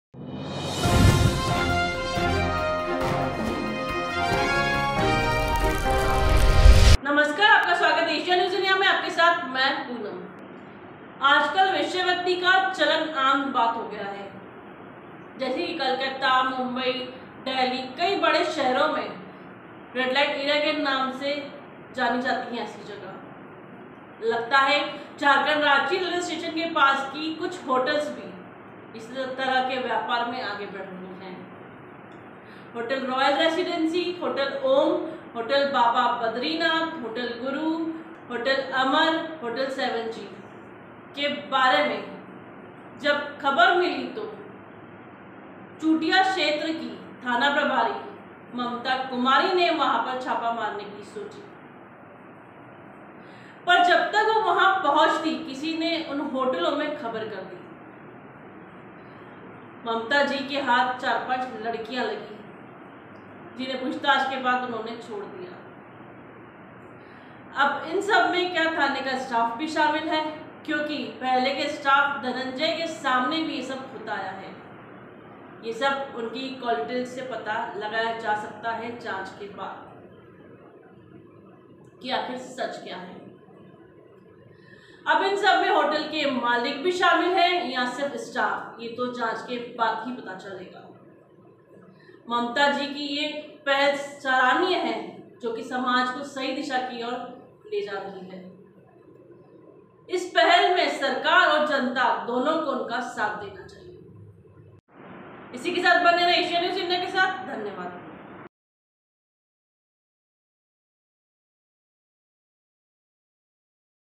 anchorpoonam रांची पुलिस ने की कारवाही मचा होटलो में हड़कंप
About this video:रांची पुलिस ने की कारवाही मचा होटलो में हड़कंप